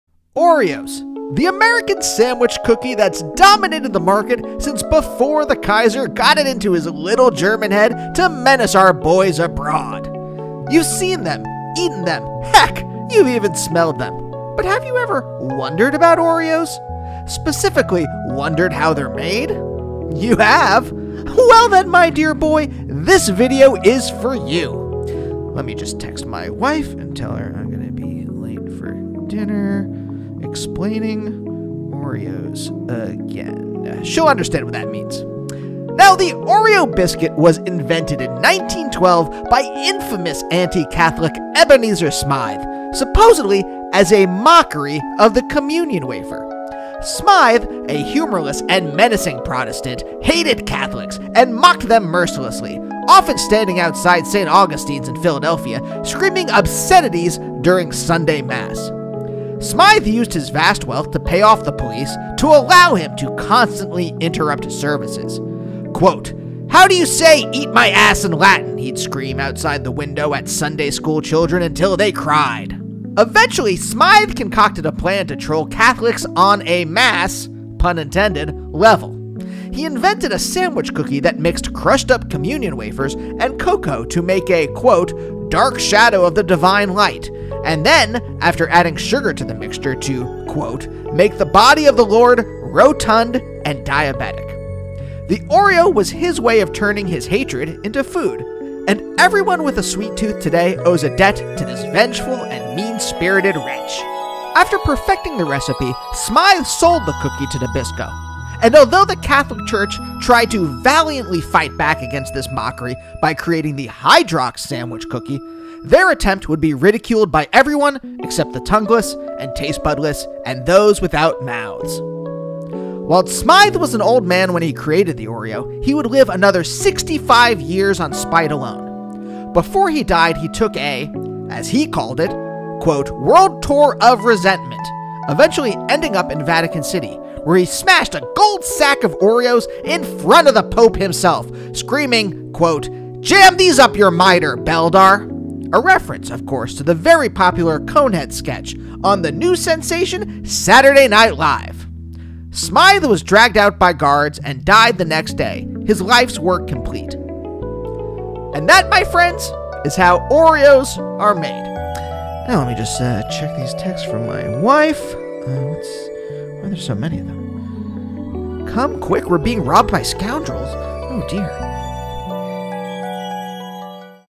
How Oreos Are Made
An instructional video from 1979 about how delicious Oreo cookies are made!